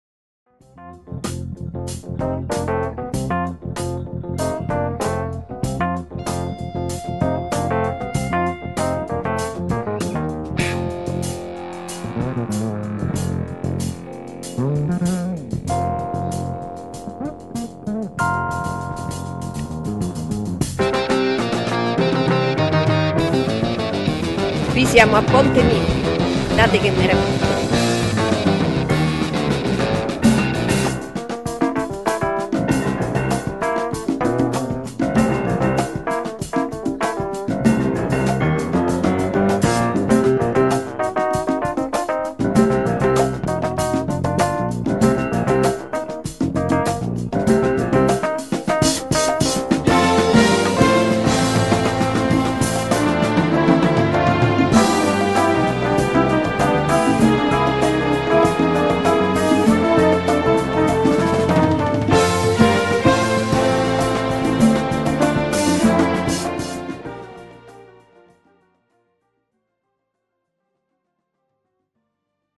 Rhapsody in Bike ...
Roma in bicicletta 🚲 per gli amanti delle due ruote.
Scoprire Roma pedalando: tutto un altro fascino!